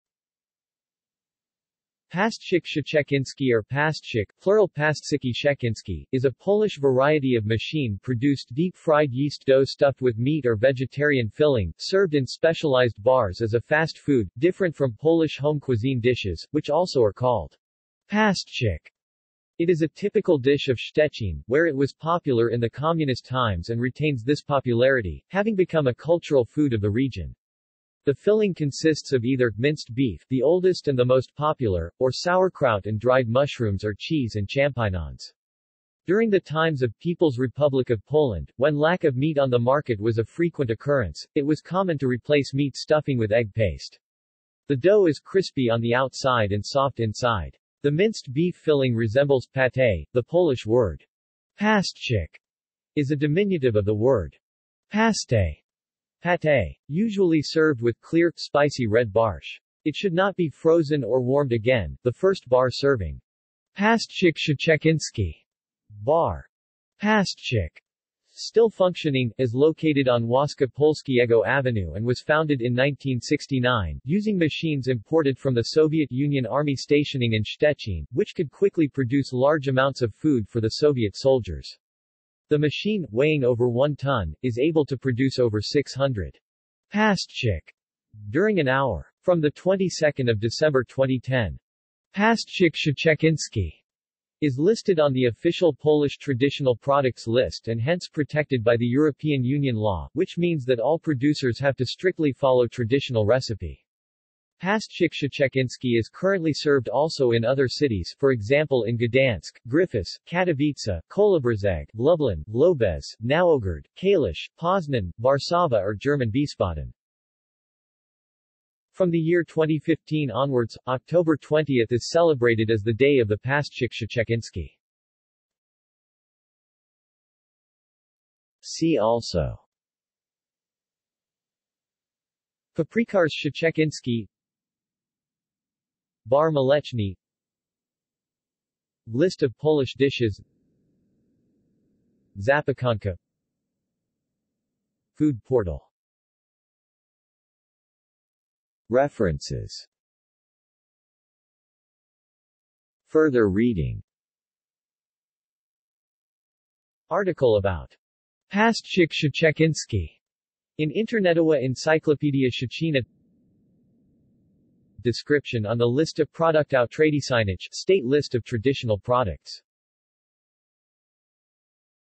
Pasztecik szczeciński | Wikipedia audio article
This is an audio version of the Wikipedia Article:
Pasztecik szczeciński

SUMMARY
Pasztecik szczeciński or pasztecik (plural paszteciki szczecińskie) is a Polish variety  of machine-produced deep-fried yeast dough stuffed with meat or vegetarian filling, served in specialised bars as a fast food, different from Polish home-cuisine dishes, which also are called "pasztecik". It is a typical dish of Szczecin, where it was popular in the communist times and retains this popularity, having become a cultural food of the region. The filling consists of either: minced beef (the oldest and the most popular), or sauerkraut and dried mushrooms or cheese and champignons. During the times of People's Republic of Poland, when lack of meat on the market was a frequent occurrence, it was common to replace meat stuffing with egg paste. The dough is crispy on the outside and soft inside. The minced beef filling resembles pâté, the Polish word "pasztecik" is a diminutive of the word "pasztet" (pâté). Usually served with clear, spicy red barszcz. It should not be frozen or warmed again.The first bar serving "pasztecik szczeciński", Bar "Pasztecik" (still functioning), is located on Wojska Polskiego Avenue and was founded in 1969, using machines imported from the Soviet Union army stationing in Szczecin, which could quickly produce large amounts of food for the Soviet soldiers. The machine, weighing over one tonne, is able to produce over 600 "pasztecik" during an hour. From 22 December 2010, "pasztecik szczeciński" is listed on the official Polish traditional products list and hence protected by the European Union law, which means that all producers have to strictly follow traditional recipe. Pasztecik Szczeciński is currently served also in other cities (for example in Gdańsk, Gryfice, Katowice, Kołobrzeg, Lublin, Łobez, Nowogard, Kalisz, Poznań, Warszawa or German Wiesbaden).
From the year 2015 onwards, October 20 is celebrated as the Day of the pasztecik szczeciński.